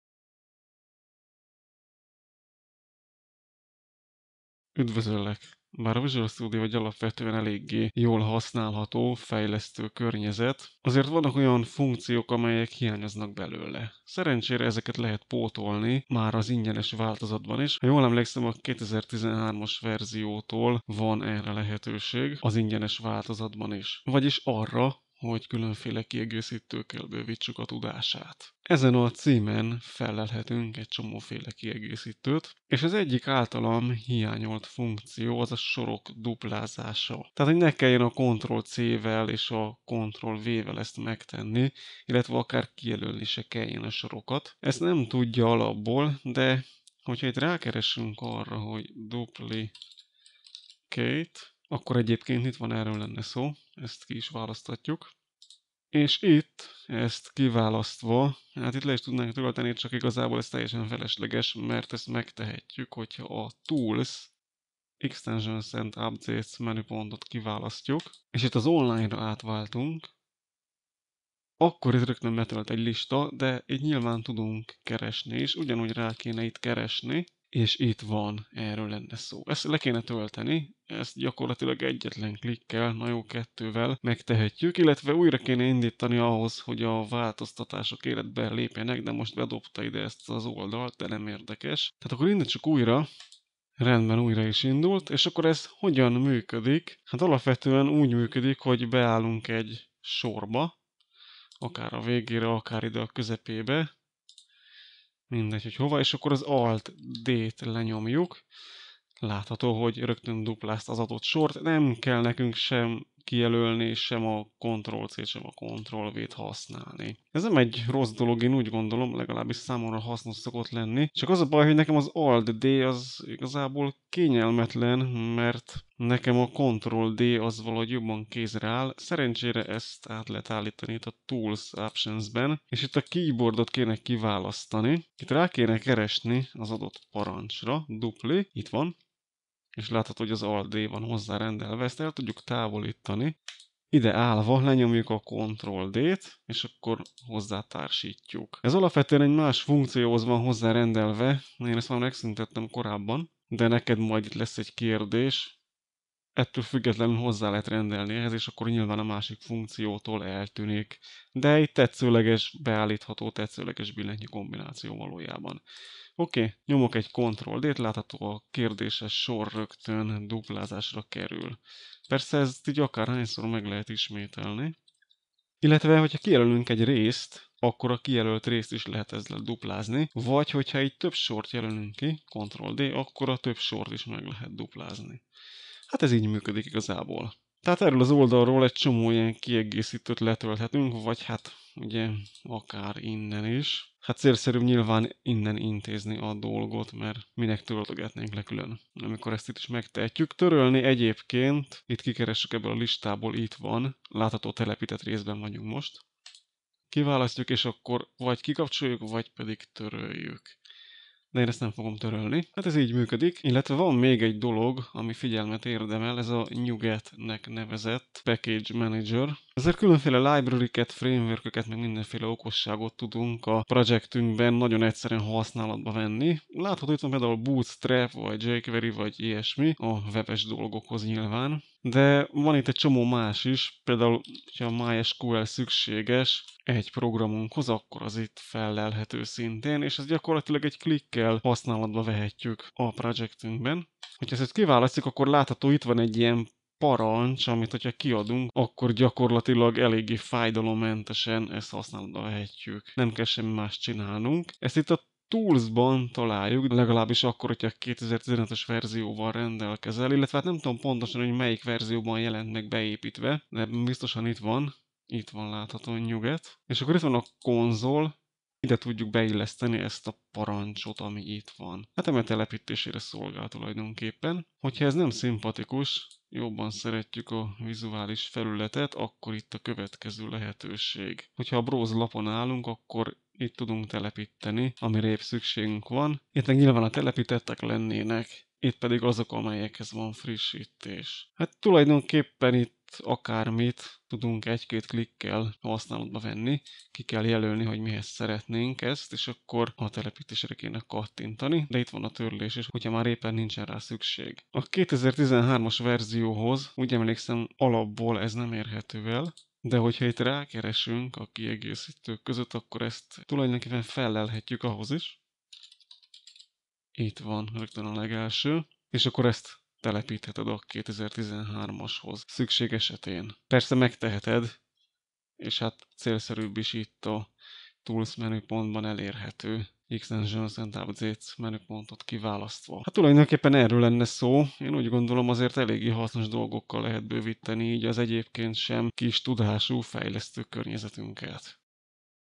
Így bővítheted a Visual Studio-t nagyon hasznos funkciókkal
A Visual Studio egy nagy tudású fejlesztő környezet, azonban vannak olyan hasznos funkciók, amelyekkel nem rendelkezik. Szerencsére ezen az ingyenes verzióban is lehet már segíteni. Hogyan? Azt nézzük meg ebben a videóban.